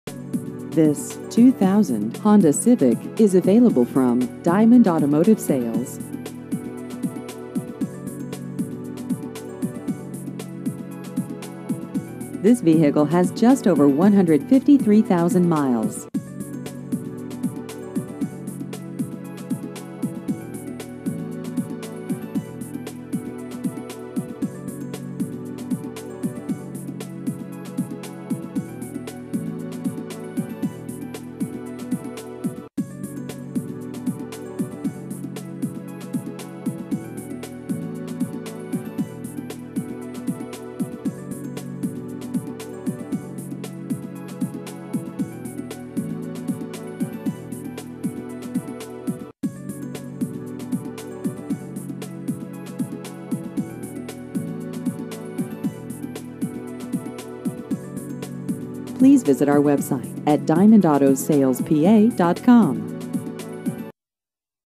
2000 Honda Civic Used Cars Glenside PA
This 2000 Honda Civic is available from Diamond Automotive Sales.